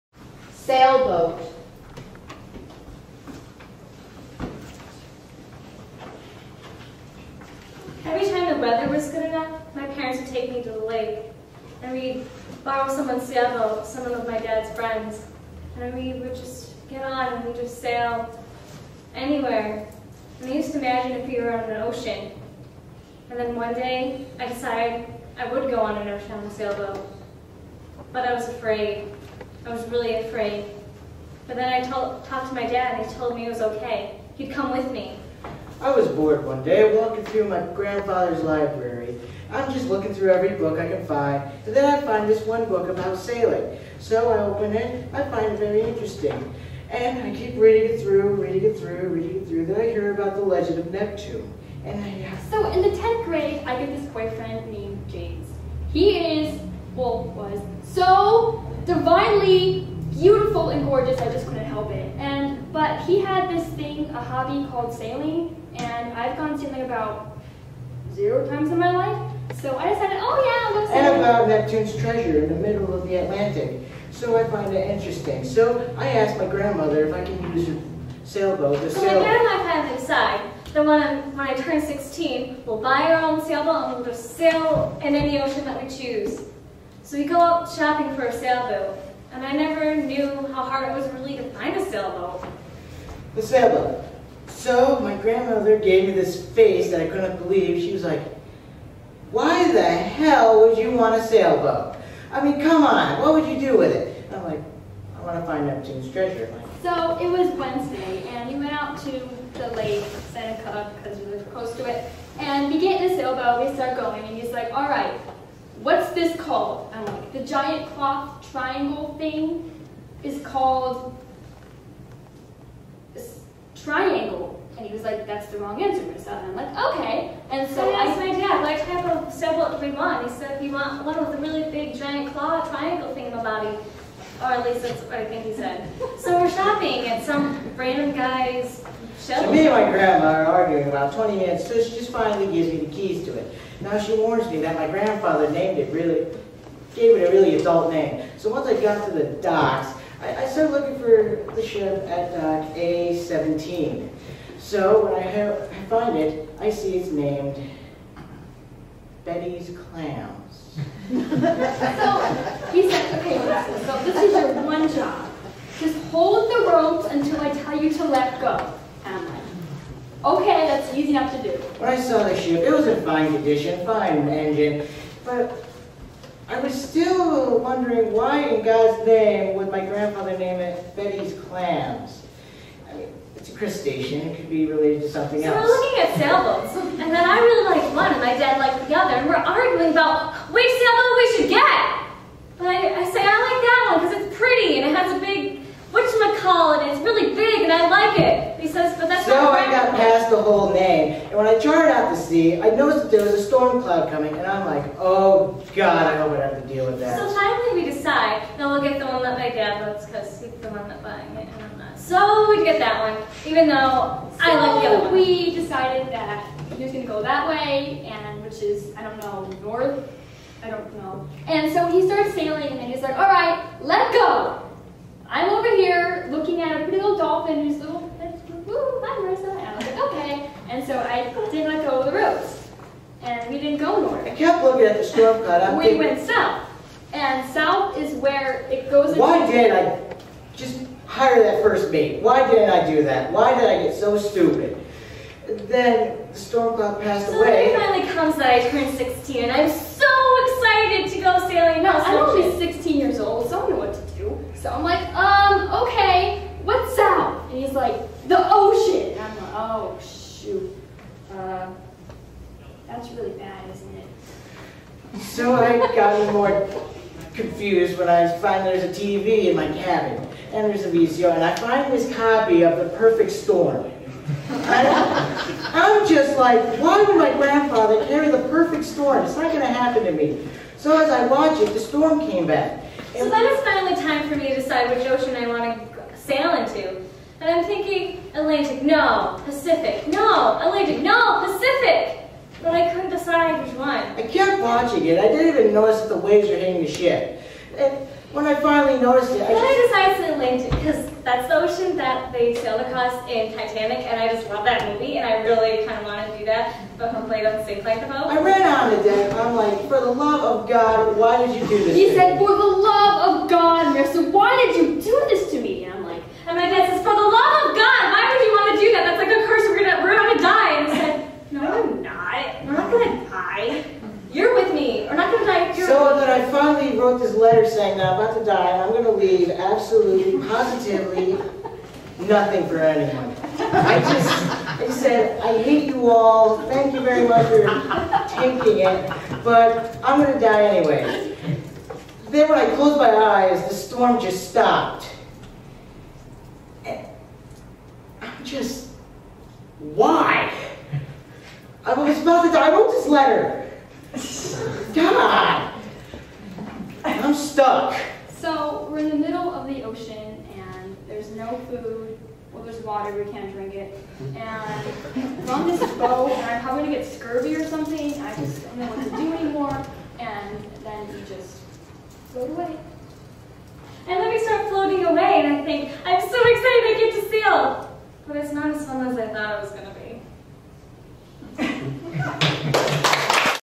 Sailboat
Corning-Painted Post East High School students tell improvised "Interrupted/Interconnected Stories" in a culminating performance after a three-week residency. Featuring Brendan Finley, Marissa Miller and Amanda Oggeri.